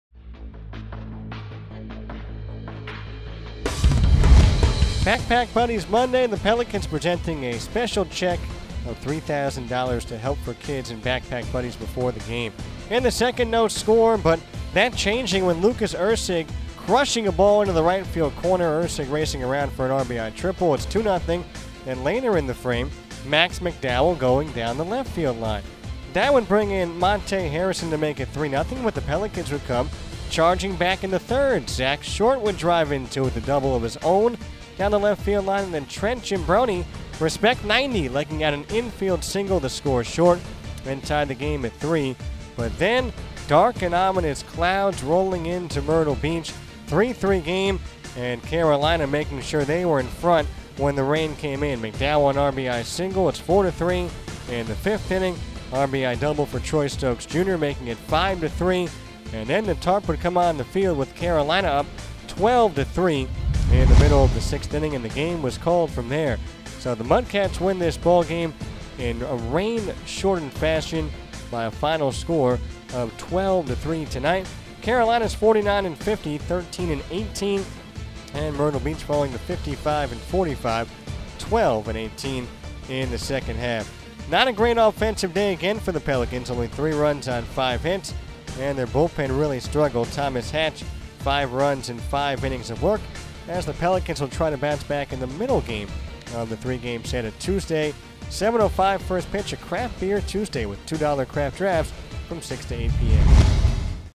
7-24-17 Highlights
The Pelicans and Mudcats had their opening game spoiled by rain. The game would be called after the sixth with the Mudcats earning the victory 12-3.